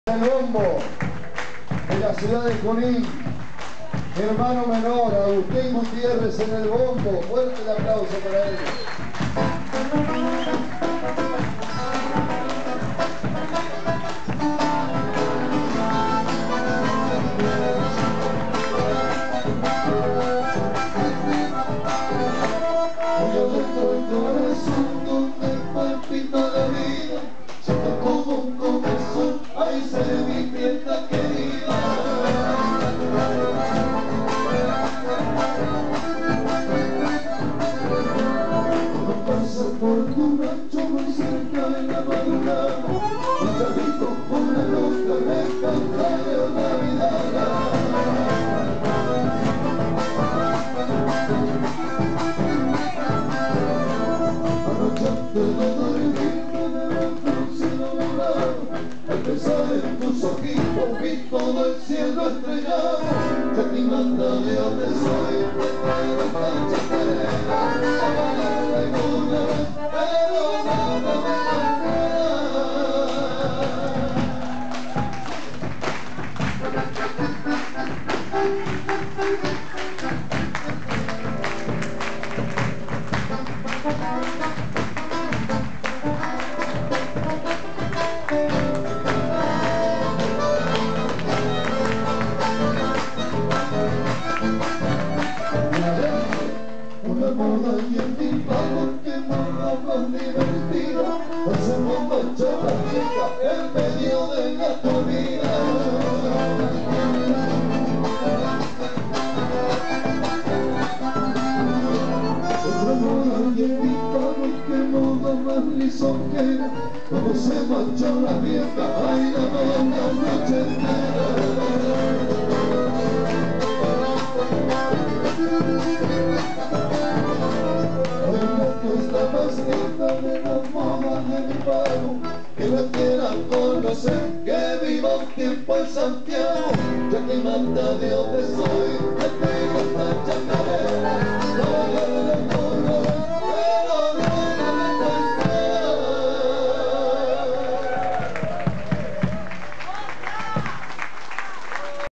"LOS GRINGOS" en el FOGÓN CRIOLLO de Cosquín 2019
Sergio Recúpero y Damián Gutierrez, a pura chacarera en el fogón criollo de Av. San Martín.